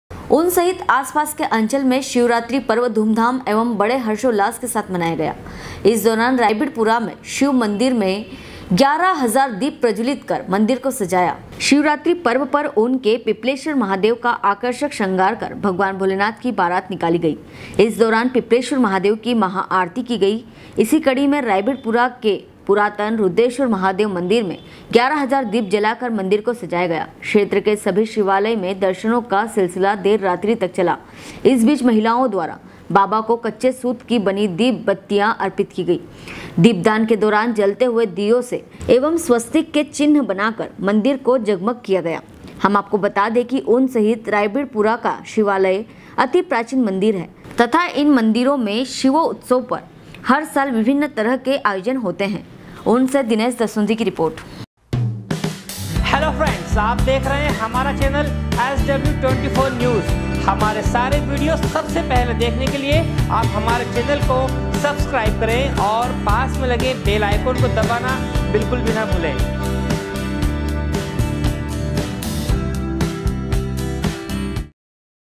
SW 24 News Madhyapradesh
ऊन सहित आसपास के अंचल में शिवरात्रि पर्व धूमधाम एवं बड़े हर्ष के साथ मनाया गया इस दौरान रायबिड़पूरा के शिव मन्दिर में 11 हजार दीप प्रजल्वित कर मंदिर को सजाया। ऊँन से दिनेश दसौंधी की रिपोर्ट